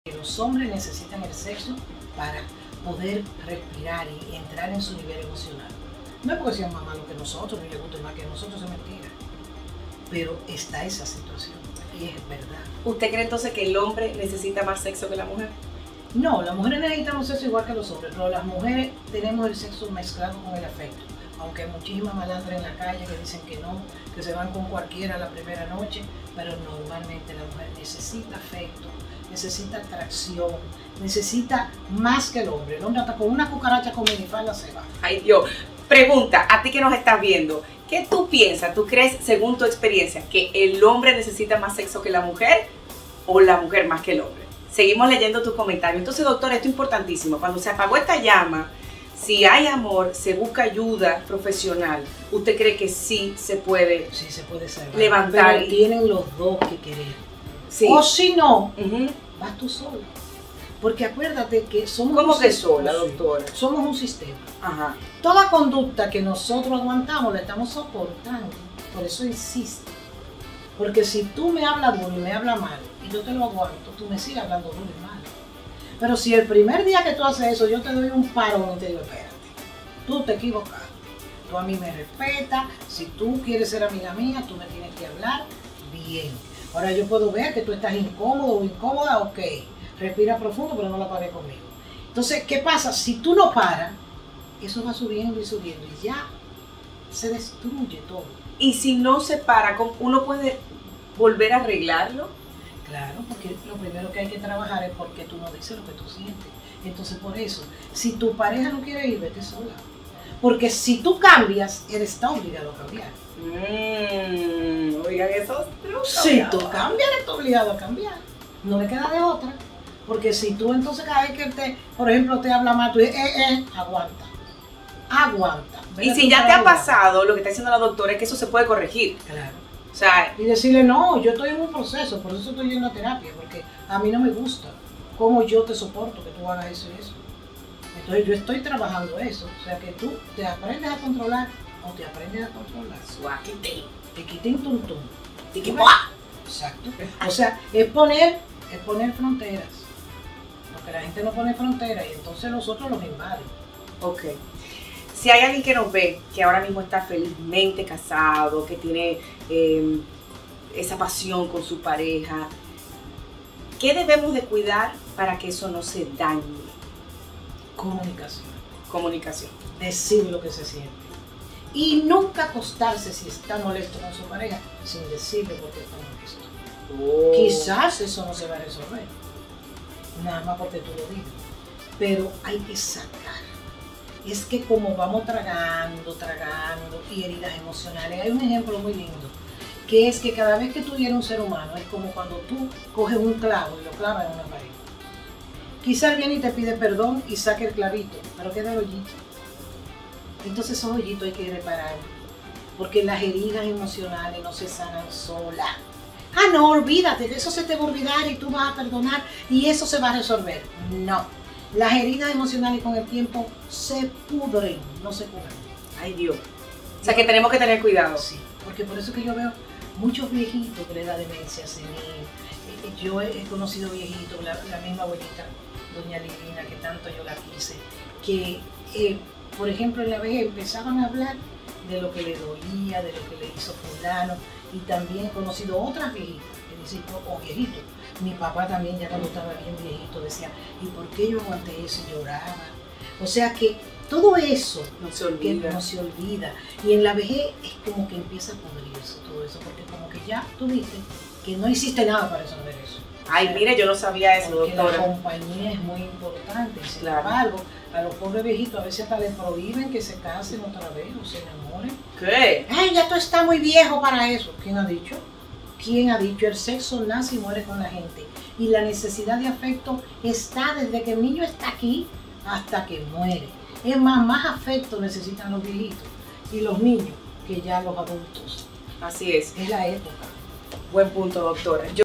El hombre ¿necesita más sexo que la mujer?
En este segmento de la entrevista que le hiciera Lourdes Stephen, la Dra. Nancy Álvarez habla de sexo y afecto, cómo se expresan en el hombre y en la mujer, de formas opuestas, y qué puede hacer la mujer para mejorar la relación, ya que los hombres no están tan dispuestos a buscar ayuda. No se pierda este video que puede mejorar su relación de pareja.